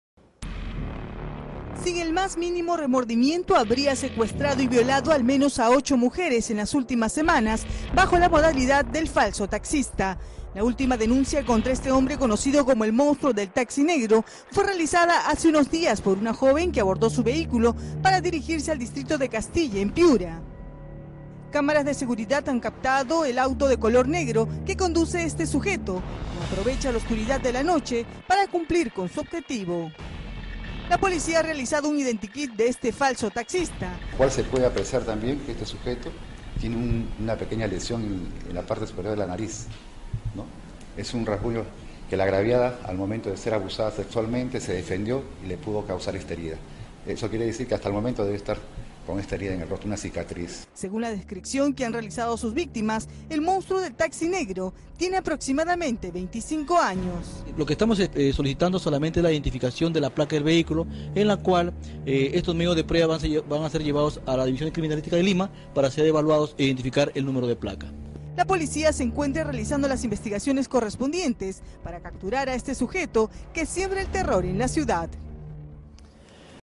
Piura: ocho mujeres fueron violadas por un falso taxista
Tras las denuncias, la policía no descarta que todas las mujeres habrían sido violadas por el mismo taxista conocido como “el monstruo del taxi negro”.